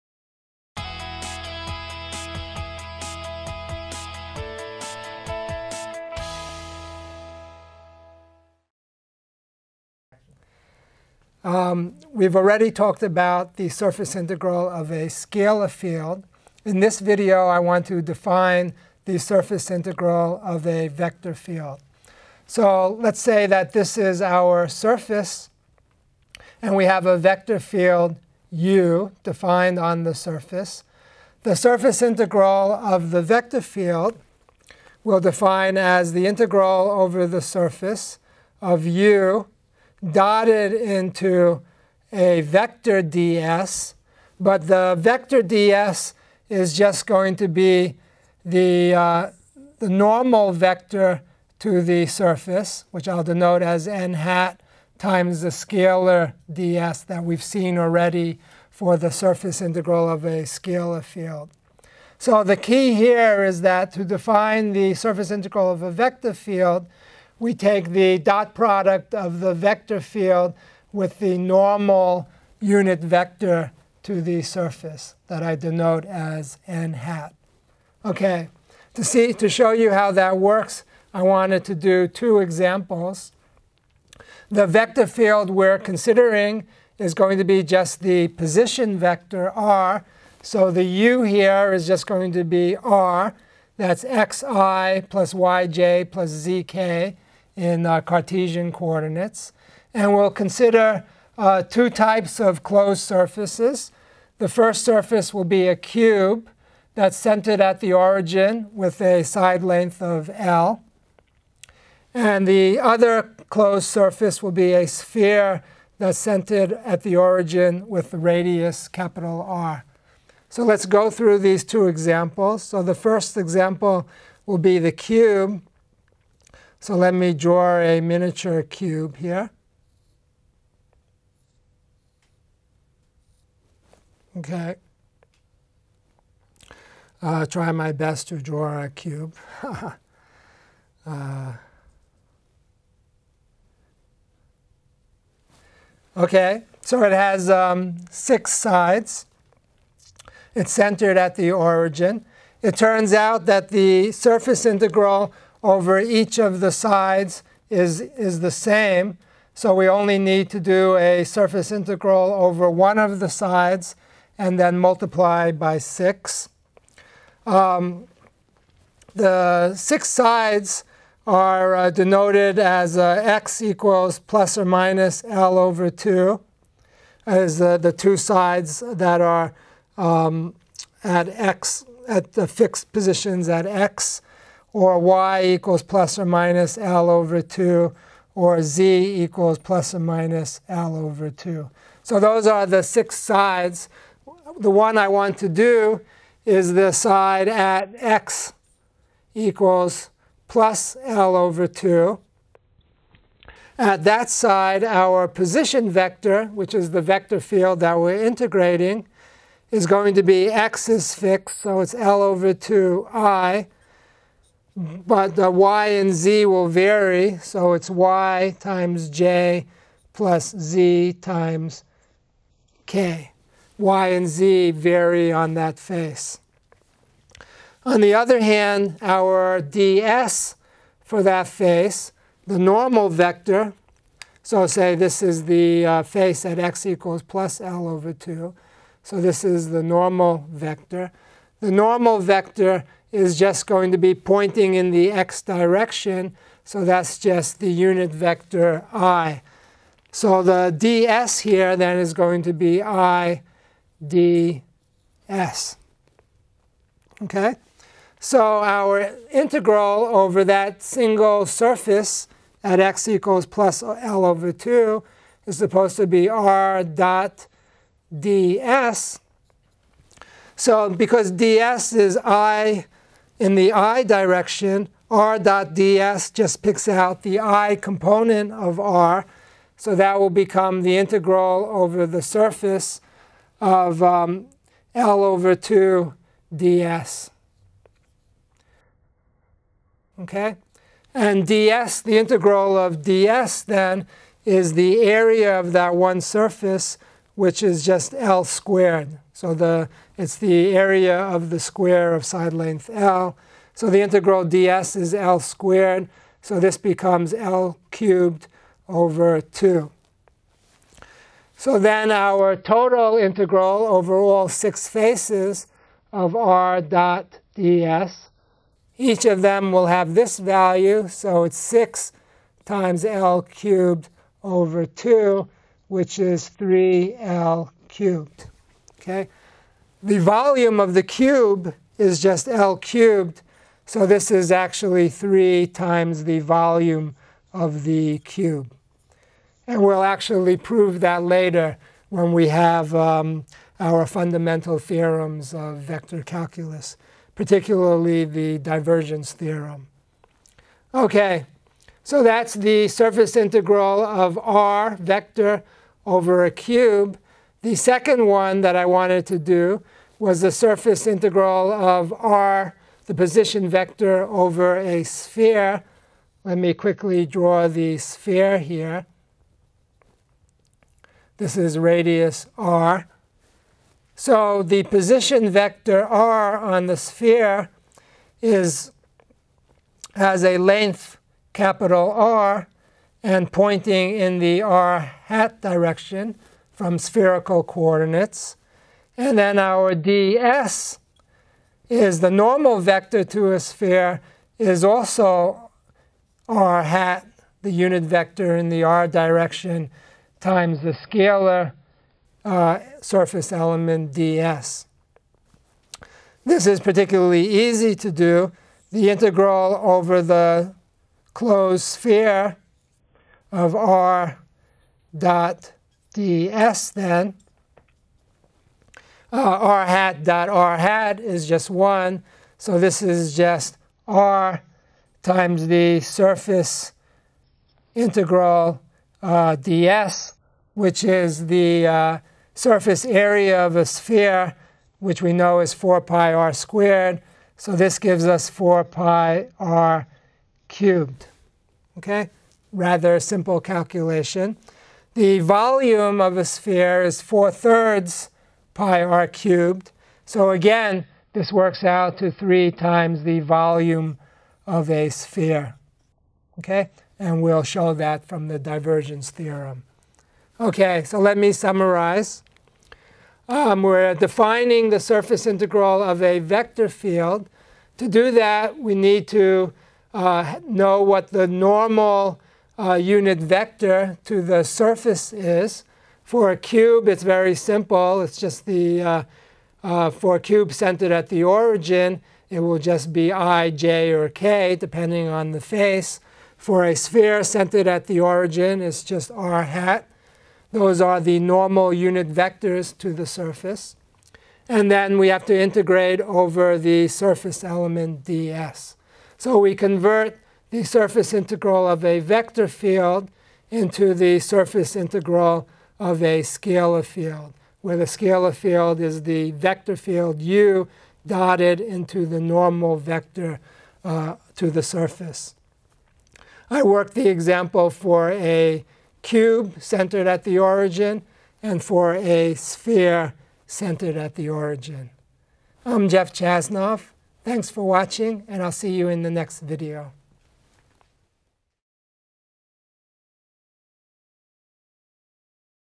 Surface Integral of a Vector Field | Lecture 41 | Vector Calculus for Engineers
How to compute the surface integral of a vector field.